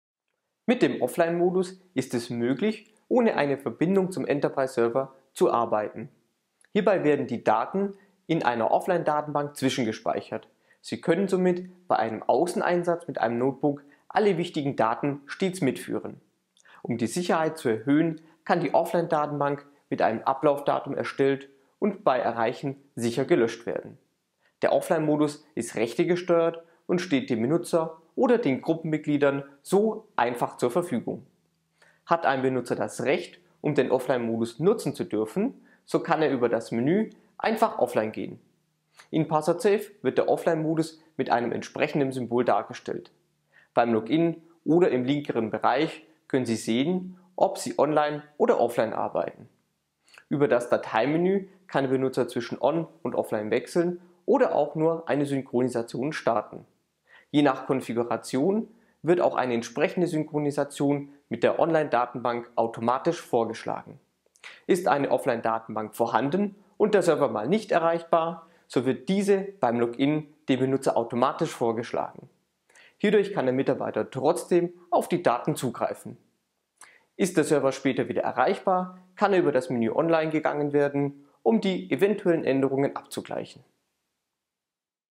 Offline-Modus - Password Safe v7
Mit dem Offline-Modus ist es möglich, ohne eine Verbindung zum Enterprise Server zu arbeiten. Hierbei werden die Daten in einer Offline-Datenbank zwischengespeichert. Sie können somit bei einem Außeneinsatz mit einem Notebook alle wichtigen Daten mitführen. Um die Sicherheit zu erhöhen, kann die Offline-Datenbank mit einem Ablaufdatum erstellt und bei erreichen sicher gelöscht werden. Der Offline-Modus ist rechtegesteuert und steht dem Benutzer oder den Gruppenmitgliedern so einfach zur Verfügung. Hat ein Benutzer das Recht, um den Offline-Modus nutzen zu dürfen, so kann er über das Menü einfach offline gehen. In Password Safe wird der Offline-Modus mit einem entsprechenden Symbol dargestellt. Beim Login, oder im unteren linken Bereich können Sie sehen, ob Sie online oder offline arbeiten. Über das "Datei" Menü kann der Benutzer zwischen On- und Offline wechseln oder auch nur eine Synchronisation starten. Je nach Konfiguration wird auch eine Synchronisation mit der Online-Datenbank automatisch vorgeschlagen. Ist eine Offline-Datenbank vorhanden und der Server mal nicht erreichbar, so wird diese beim Login dem Benutzer automatisch vorgeschlagen. Hierdurch kann der Mitarbeiter trotzdem auf die Daten zugreifen. Ist der Server später wieder erreichbar, kann über das Menü online gegangen werden, um die eventuellen Änderungen abzugleichen.